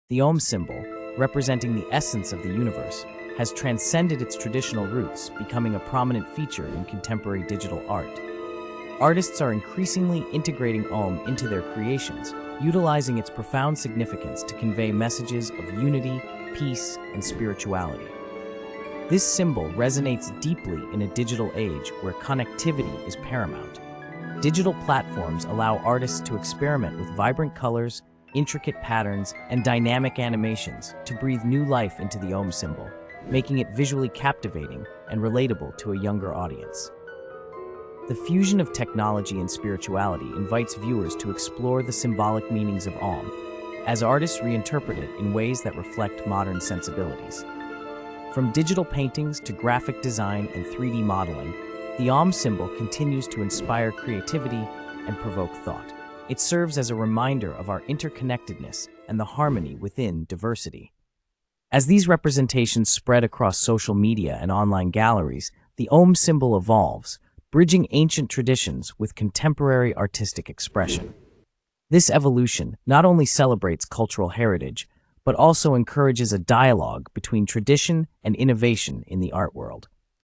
The Om Symbol: From Spirituality to Digital Art
The Om Symbol in Digital Art

Explore the stunning evolution of the Om symbol in digital art. Discover its vibrant representations and spiritual significance in contemporary culture.

The Om symbol, representing the essence of the universe, has transcended its traditional roots, becoming a prominent feature in contemporary digital art. Artists are increasingly integrating Om into their creations, utilizing its profound significance to convey messages of unity, peace, and spirituality. This symbol resonates deeply in a digital age where connectivity is paramount. Digital platforms allow artists to experiment with vibrant colors, intricate patterns, and dynamic animations to breathe new life into the Om symbol, making it visually captivating and relatable to a younger audience.

The fusion of technology and spirituality invites viewers to explore the symbolic meanings of Om, as artists reinterpret it in ways that reflect modern sensibilities. From digital paintings to graphic design and 3D modeling, the Om symbol continues to inspire creativity and provoke thought. It serves as a reminder of our interconnectedness and the harmony within diversity. As these representations spread across social media and online galleries, the Om symbol evolves, bridging ancient traditions with contemporary artistic expression. This evolution not only celebrates cultural heritage but also encourages a dialogue between tradition and innovation in the art world